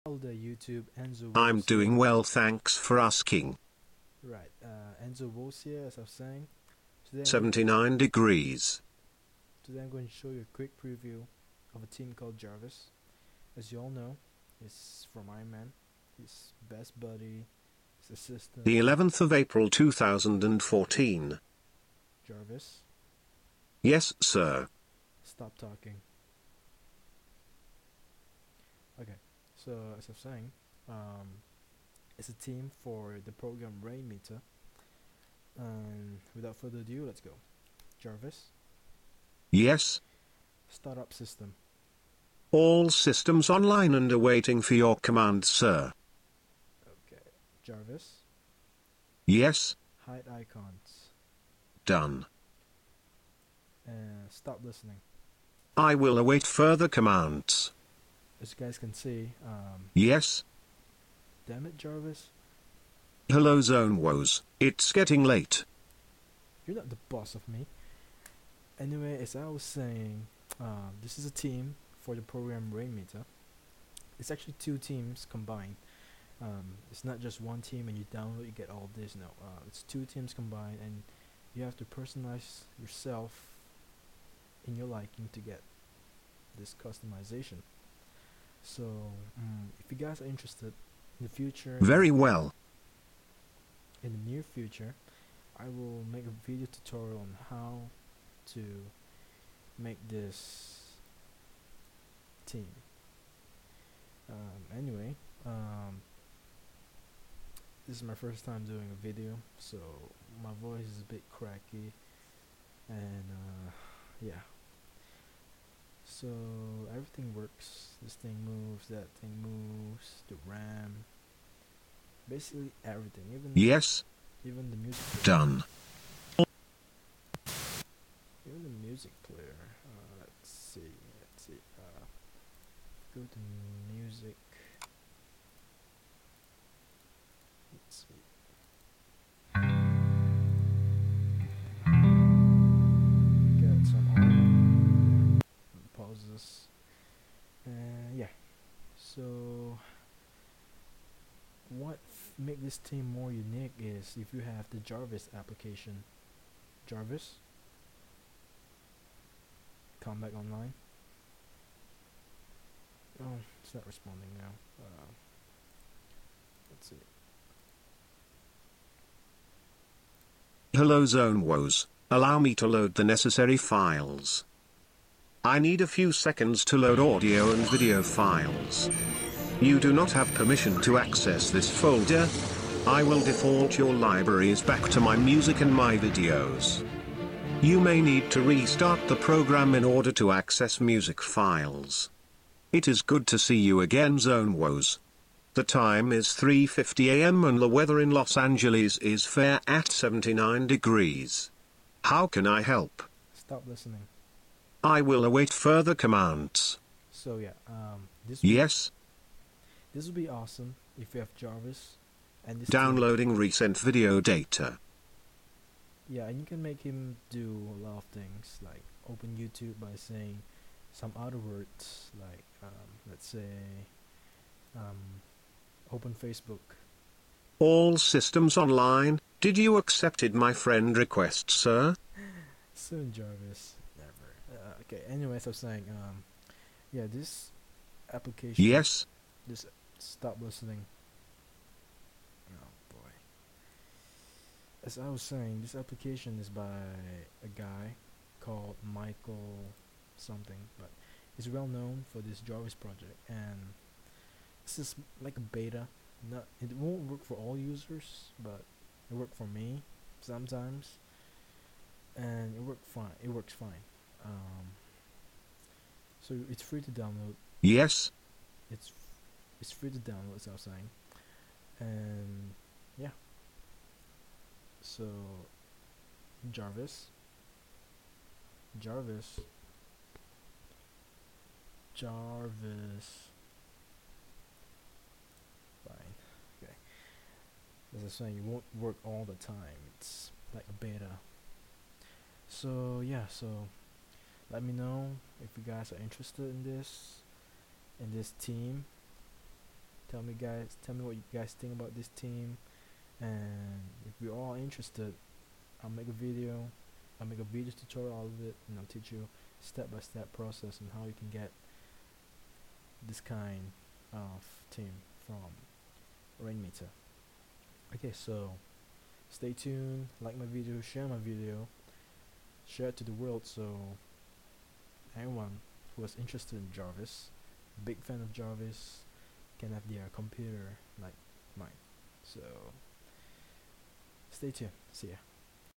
My Jarvis Rainmeter theme
This is my J.A.R.V.I.S theme from a program called rainmeter.
It also features Michael C's J.A.R.V.I.S. app.
If you guys are interested in this theme,let me know and i'll make a video tutorial for you guys:)
It's my first time making a vid,so don't hate :P